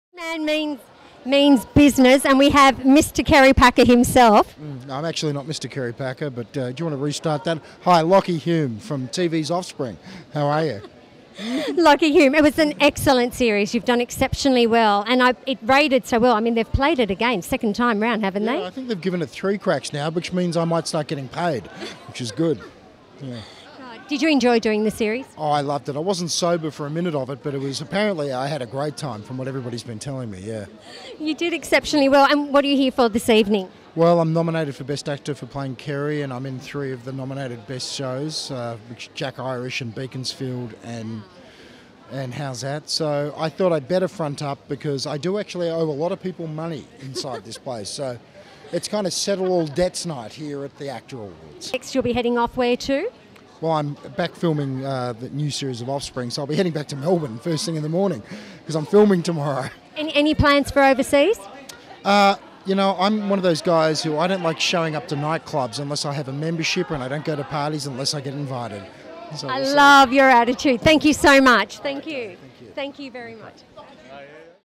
Lachy Hulme & Janette Lakiss @ AACTA Awards @ The Star, 2013
Lachy Hulme & Janette Lakiss, AACTA Awards,The Star, 2013 Lachy Hulme plays "Mr Kerry Packer" in Howzat!, also stars in Offspring, Jack Irish, Beaconsfield Mine Disaster Telemovie, Very witty, clever & fun guy!

Filmed by Maria Goretti